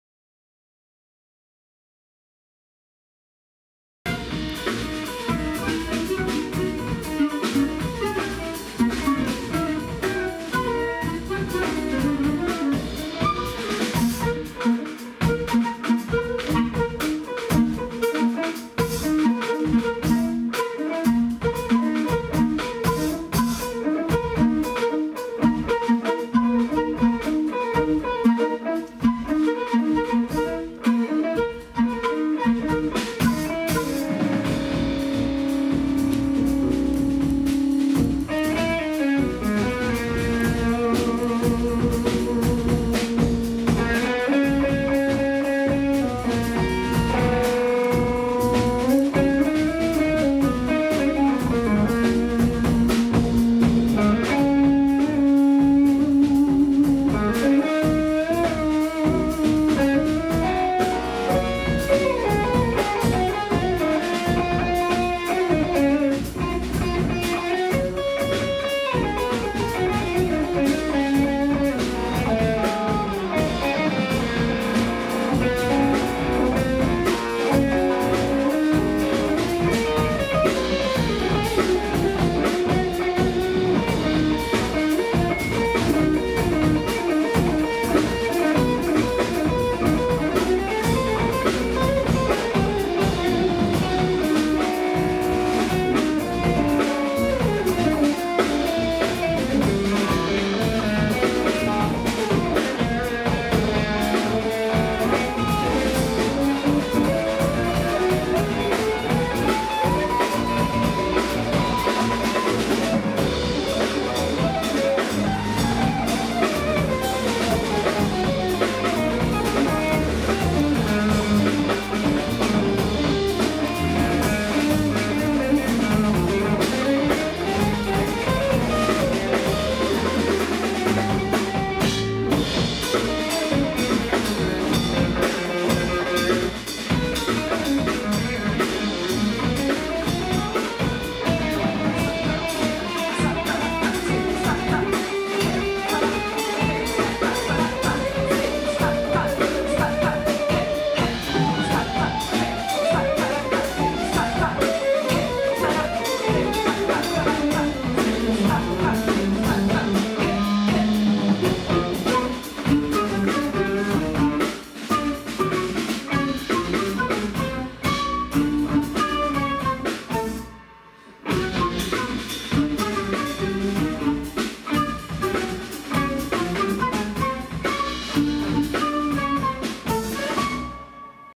Aram, l'un des quatre groupes en lice pour le tremplin Tonnerre de Jazz à Billère
Aram, l'un des quatre groupes en lice pour le tremplin Tonnerre de Jazz à Billère. Votez pour votre favori sur sudouest.fr